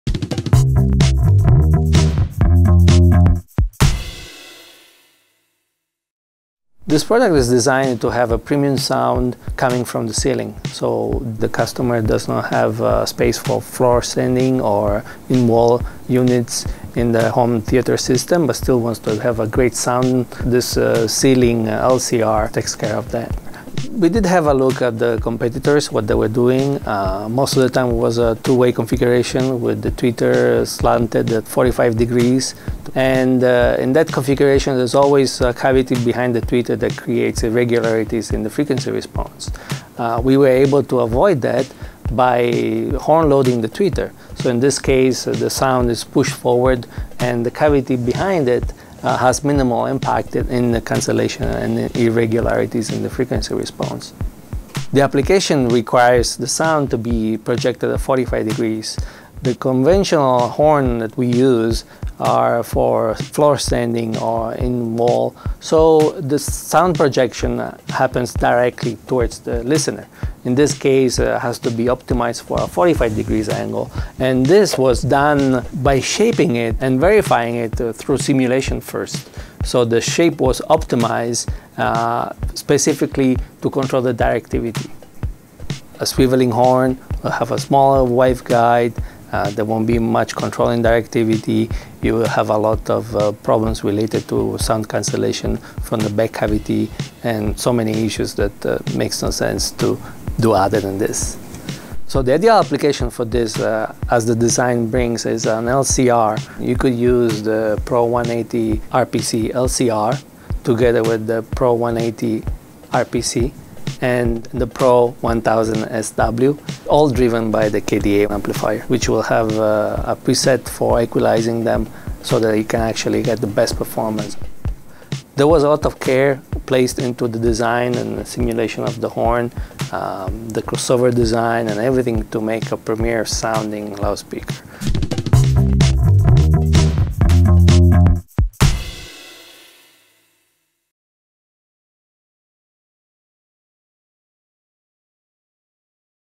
Beyond the Specs with the Pro 180RPC LCR In-Ceiling Speaker
Meet Riccardo Balistreri, Acoustics and Design Engineer, for the PRO 180RPC LRC speaker. Here, he'll walk us through the key features and technologies behind this amazing product.